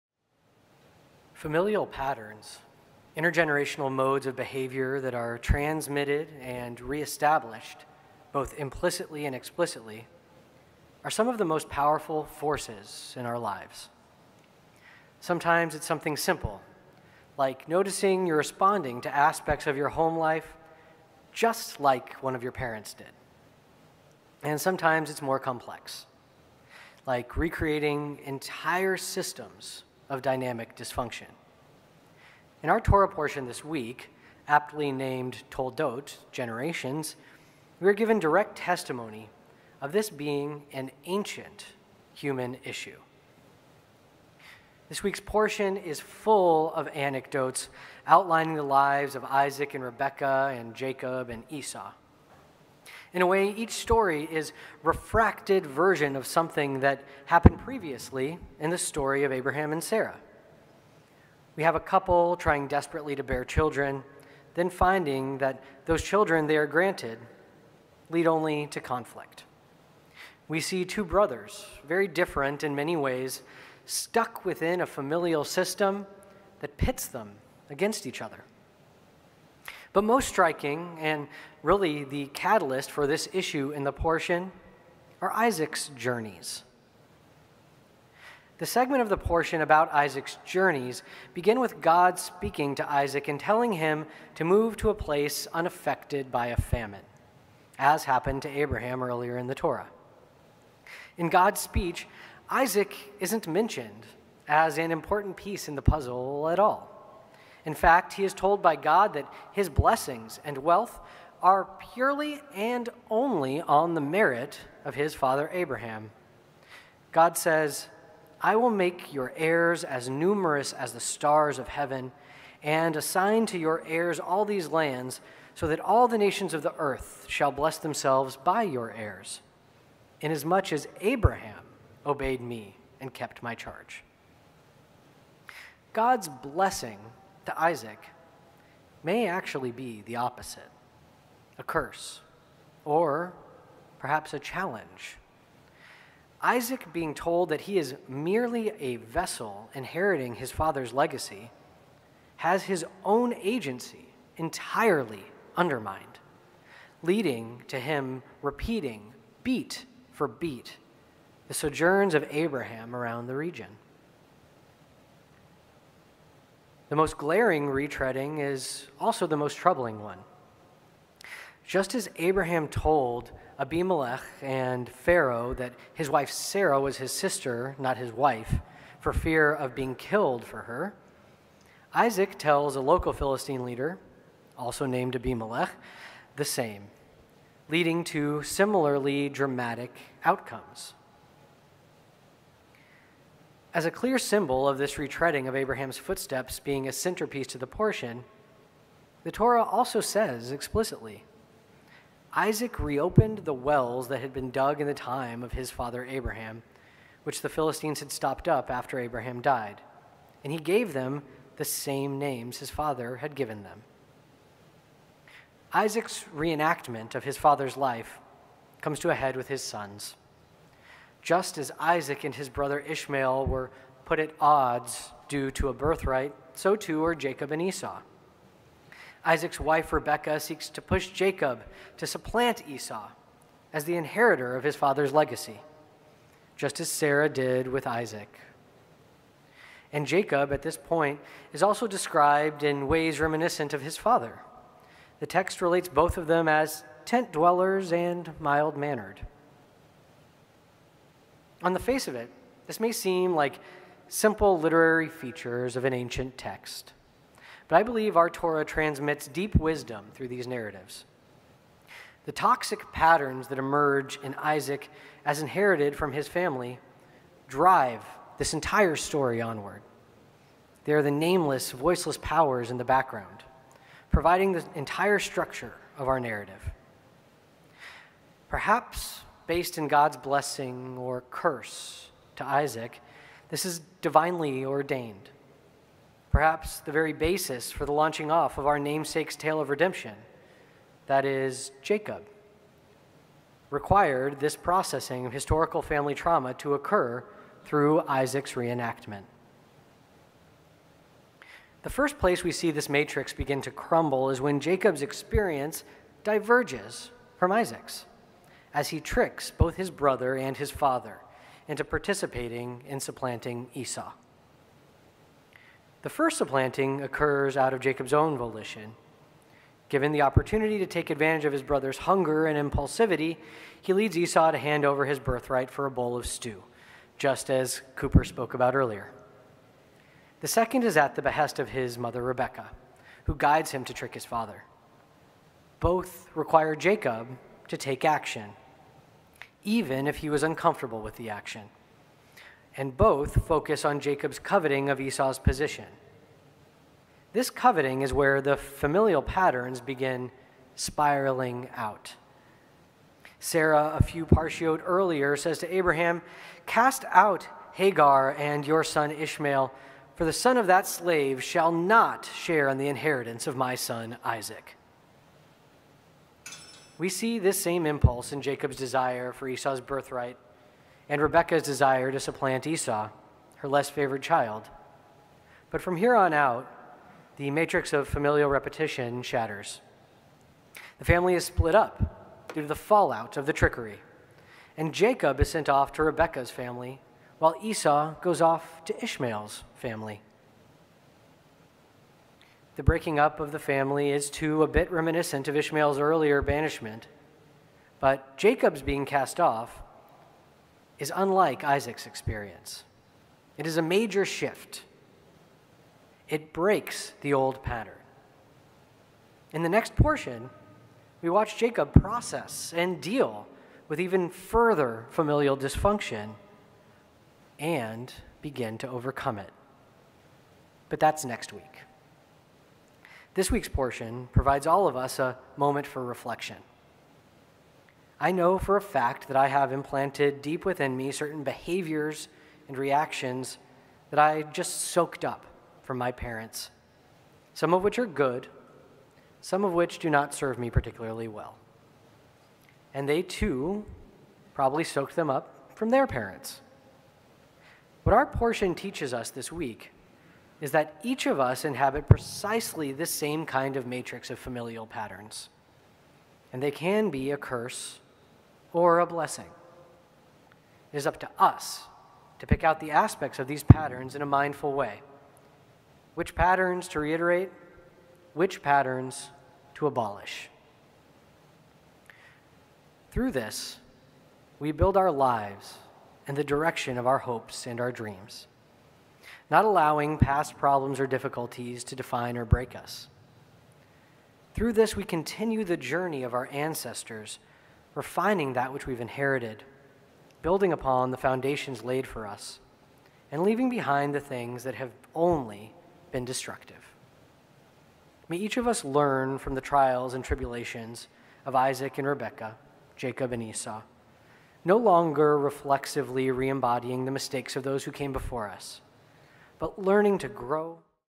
Toldot 2021: The Power of Familial Patterns
Our Torah teaches us that familial patterns of behavior can govern the future of entire peoples, but we also have the power to move beyond them. In this week's Torah portion, we see an Isaac stuck deeply in the patterns of his past - what can we learn from his struggles?